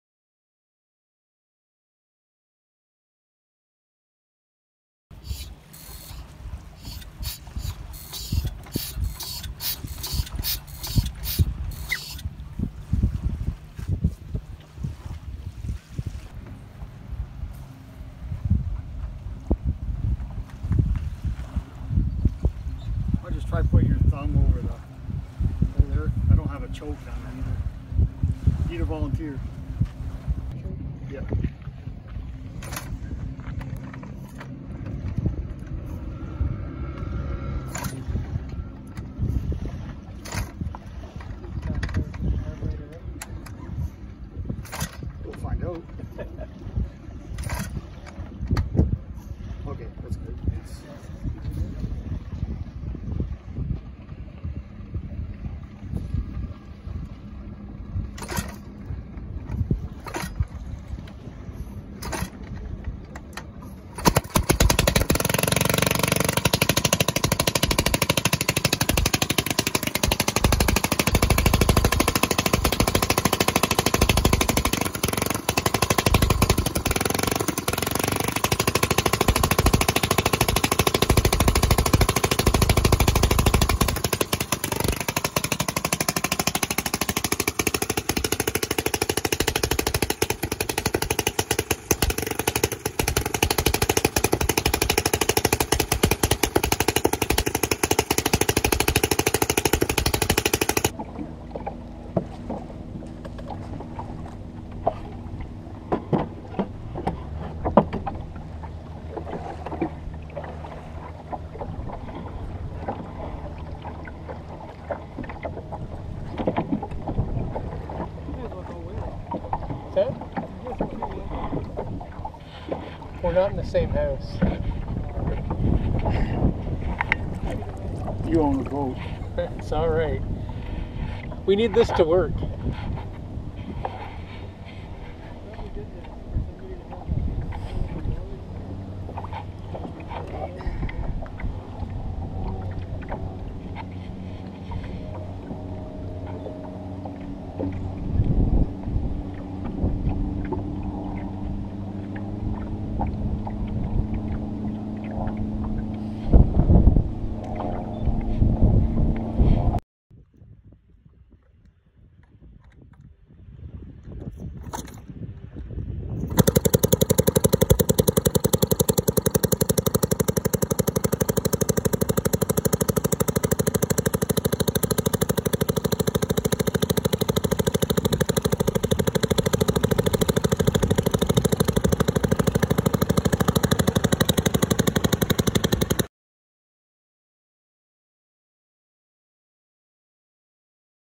1909 Waterman Coughing Sarah Rowboat Motor RBM
Running a 1909 Waterman Coughing Sarah Antique Outboard. This rowboat motor (RBM) is an example of the first successfully commercial outboard motor. The Waterman "Coughing Sarah" as it is commonly known was originally fashioned from a Curtis motorcycle engine and first made available for sale in 1906. This 1909 version is slightly more refined and predates the brass era.

Follow us on:
FACEBOOK;   Upper Canada Chapter AOMCI
TWITTER;   uccaomci
INSTAGRAM;   uccaomci
TUMBLR;   uccaomci